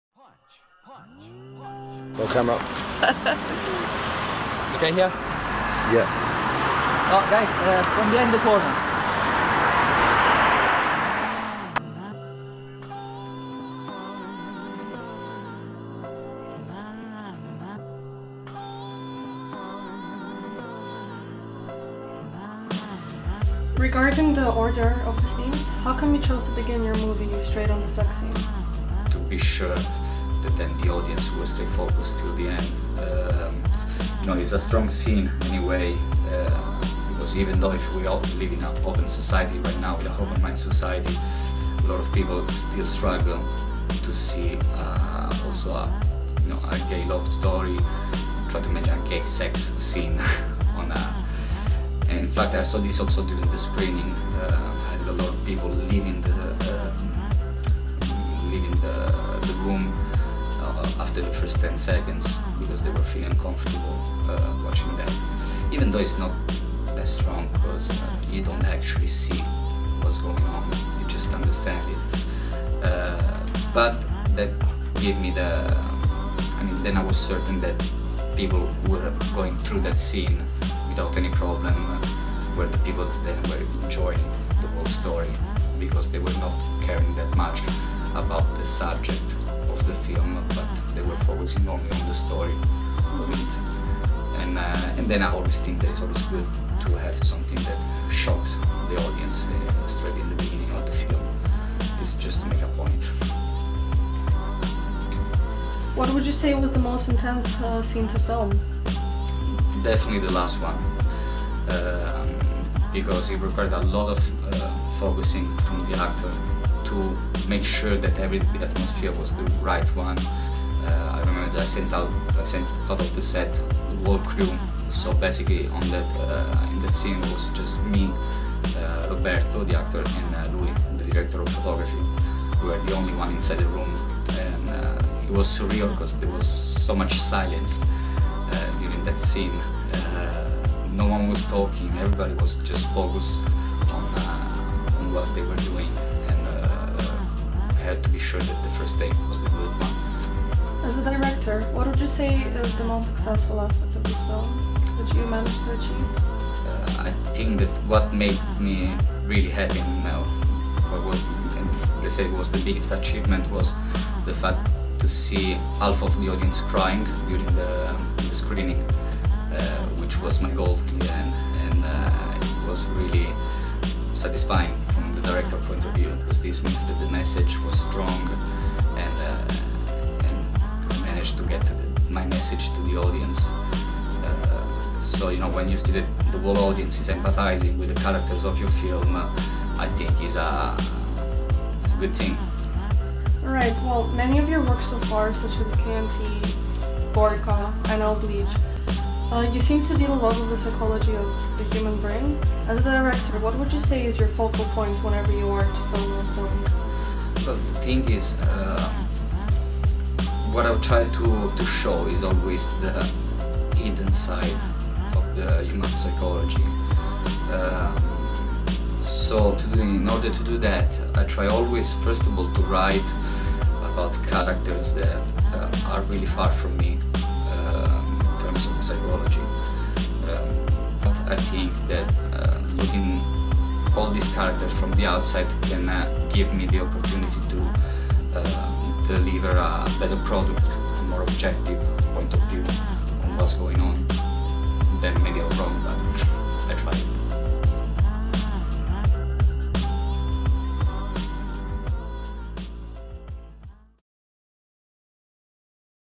Interview with the director - Episode 1 - Claudio Ravanelli
Interview with Claudio Ravanelli,the director of the award winning short film K&P (2013)